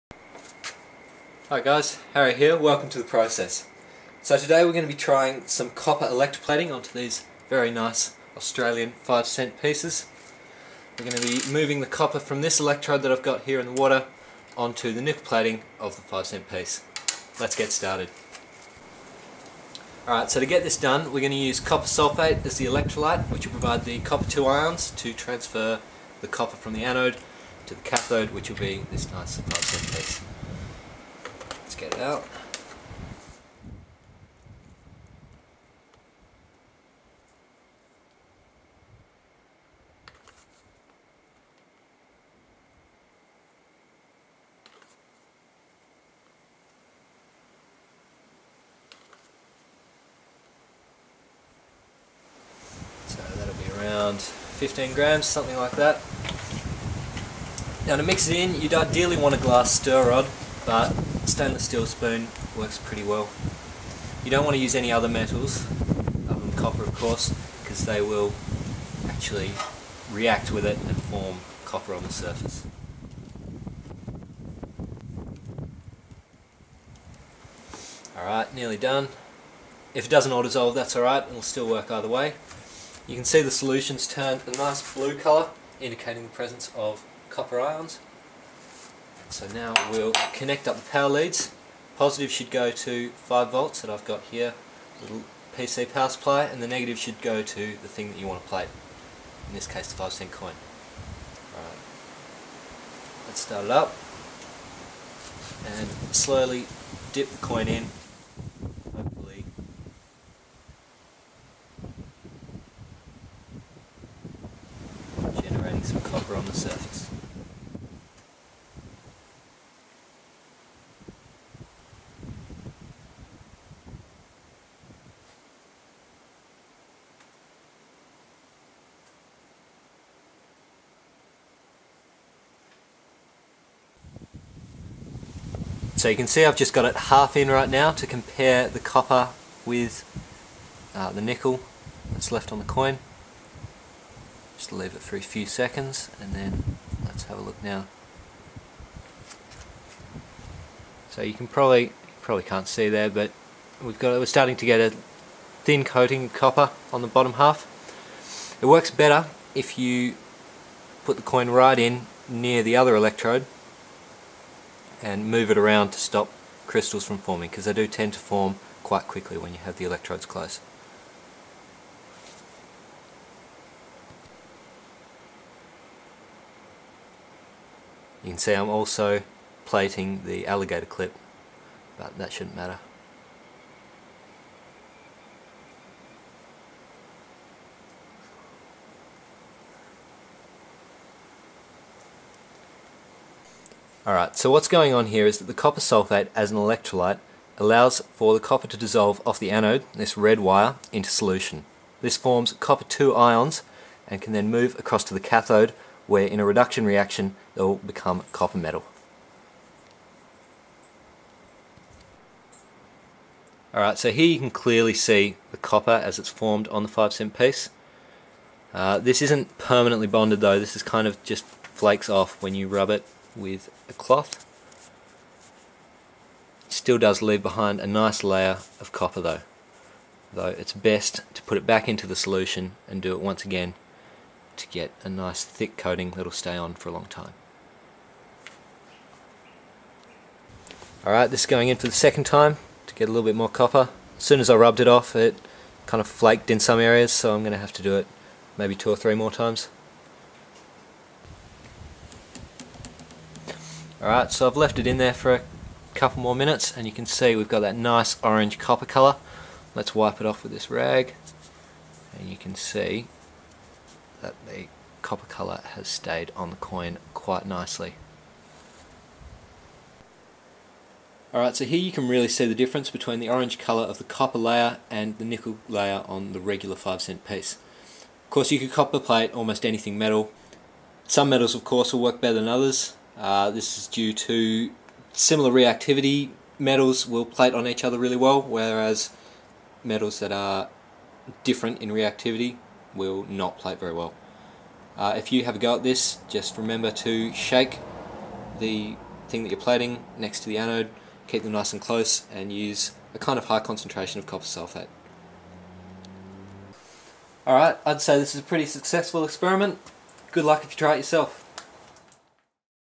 Copper Plating
Demonstrating an easy method of electroplating copper onto metal surfaces.